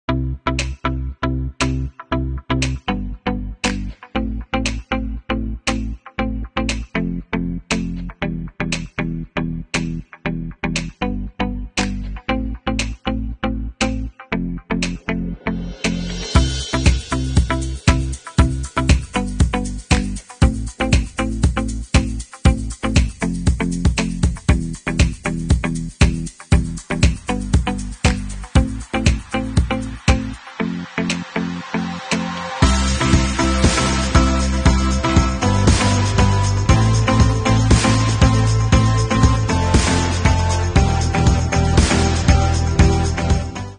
Men's Snip Toe Cowboy Boots Burnished Brown (H50030)
Experience exemplary craftsmanship and comfort with this classic cowboy boot, handcrafted in Leon, Guanajuato, Mexico with genuine, burnished leather. The brown leather is complemented by traditional snip toe and western stitching on the shaft and foot. Let these boots complete your authentic Western look.